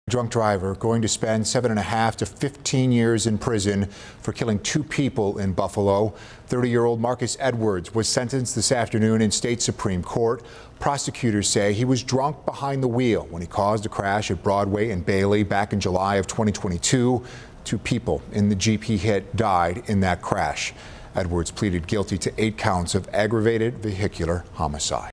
Drunk driver sentenced in connection to deadly crash on Bailey Avenue in July 2022
The Erie County District Attorney's Office announced a drunk driver was sentenced in connection to a deadly crash on Bailey Avenue in July 2022.